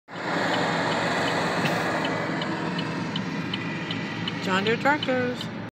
John Deere Tractors 💚 💛
John Deere Tractors, they are out mowing grass on the sides of the highways.